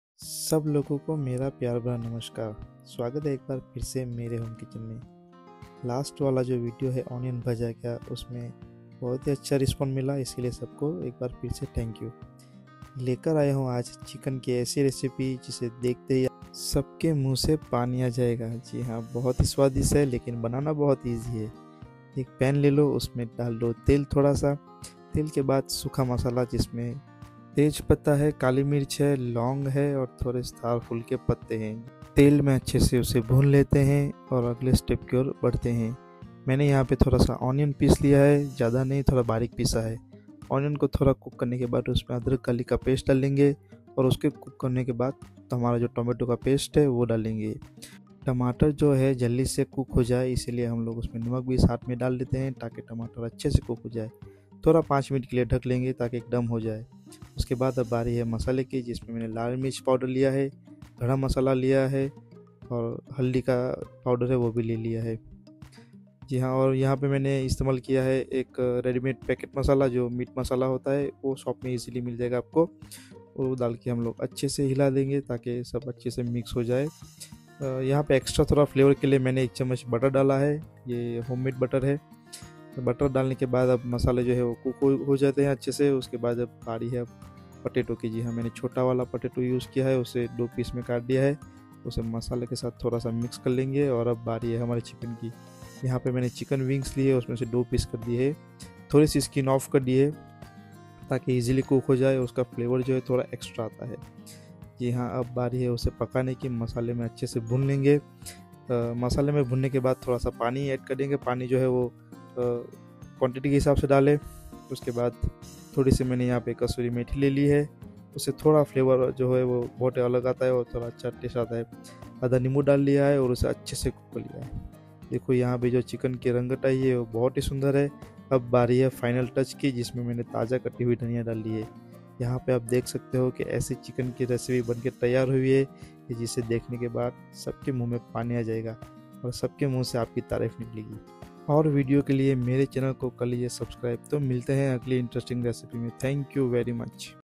CHICKEN MASALA TODAY SPECIAL
A simple and easy recipe for who loves a chicken 🐓 I made in my own way so easy and delicious 😋